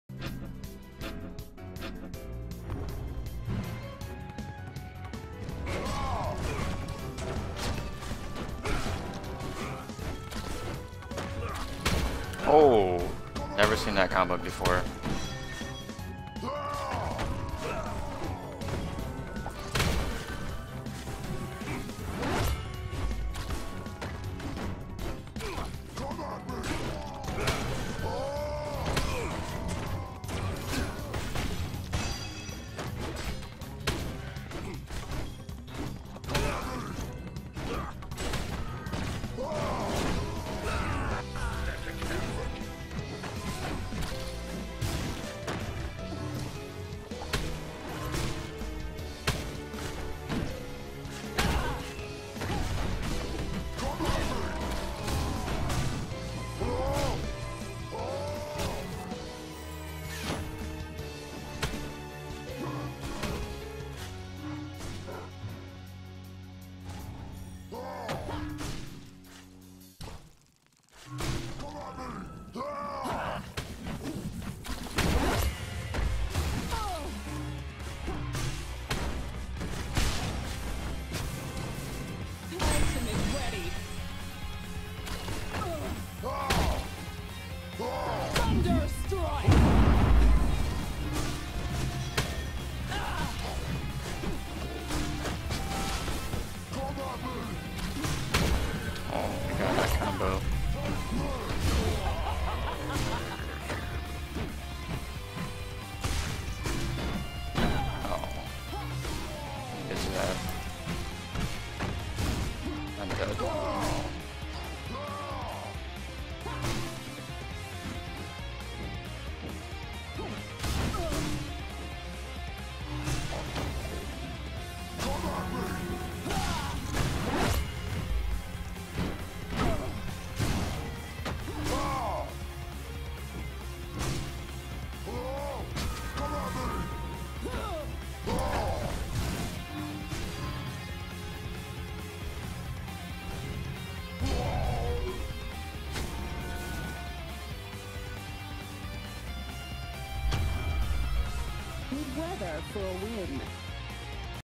Impressive 1v2 dual box against 2 GC level players
Happened October 2017. Took a round against 2 GC level players with my dualbox team -- Watch live